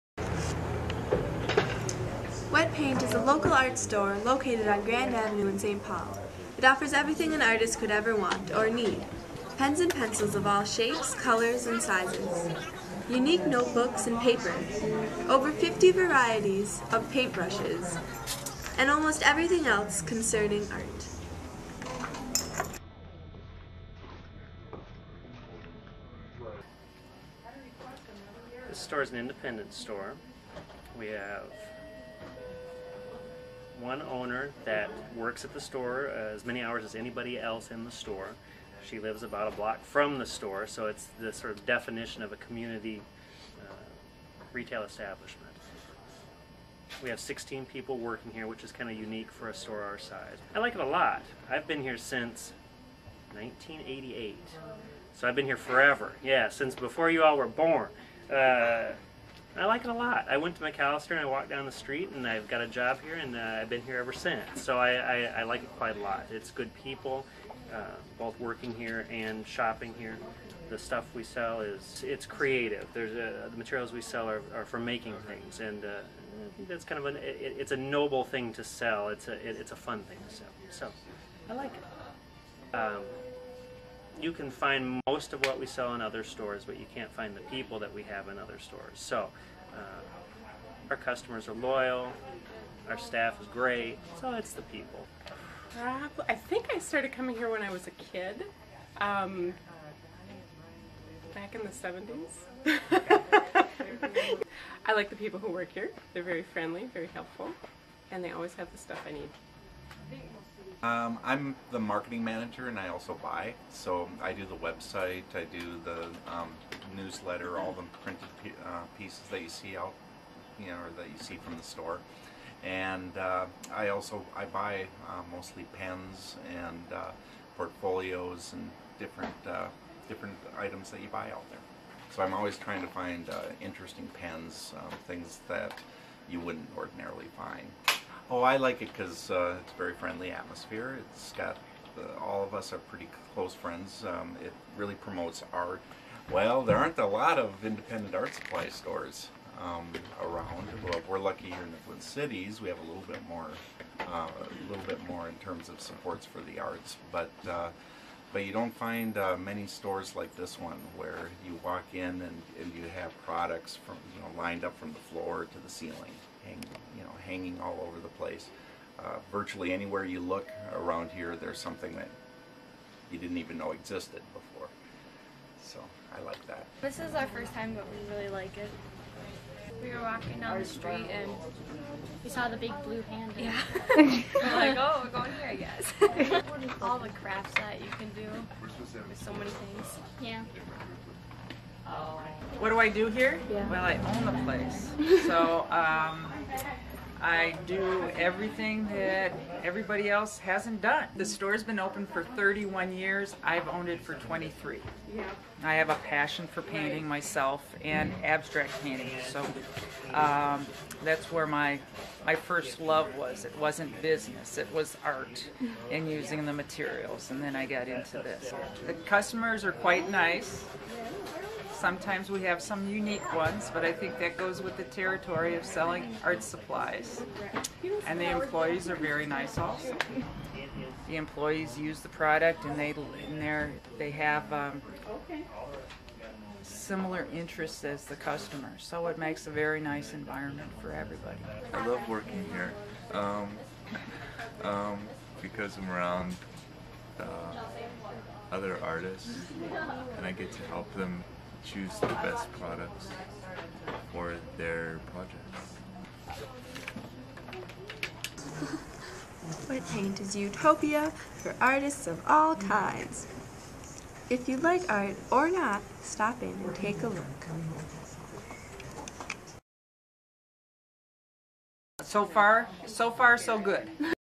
Wet Paint - Art Supply Store - St. Paul Minnesota
A video tour of our locally owned and operated art store on Grand Avenue in St.Paul.